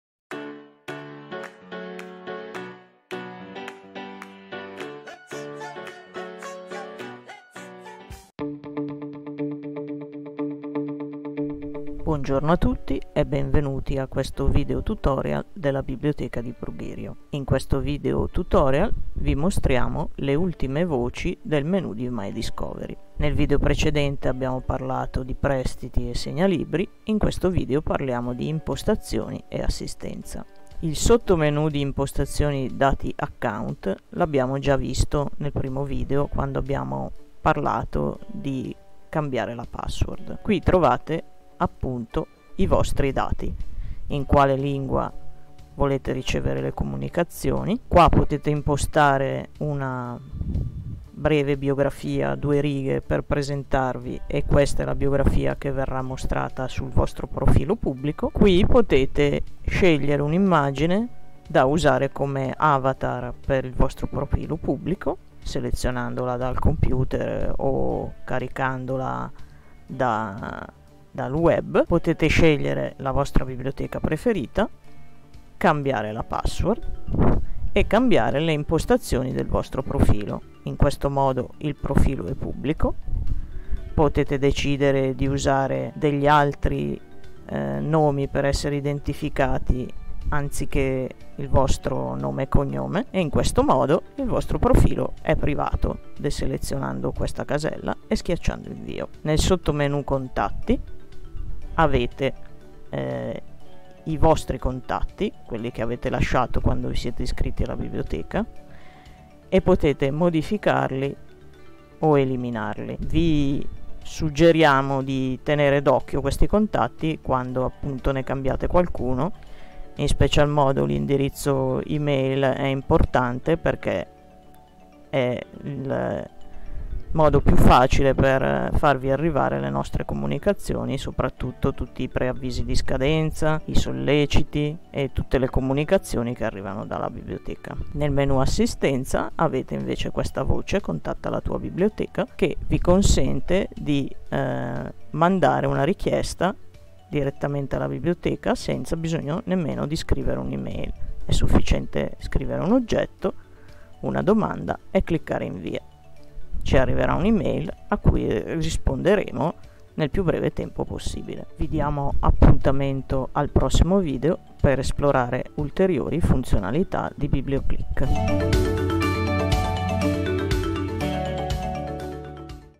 Il menu di MyDiscovery: impostazioni (dati account) e assistenza (contatta la tua biblioteca)
In questo video continuiamo ad illustrarvi le funzionalità del menu di MyDiscovery, una volta che vi siete autenticati sul sito Descriviamo il menu Impostazioni (dove potete modificare i vostri dati, i vostri contatti e recapiti, la vostra biblioteca preferita e rendere pubblico o privato il vostro profilo) e il menu Assistenza (in cui potete contattare la biblioteca che avete impostato come preferita).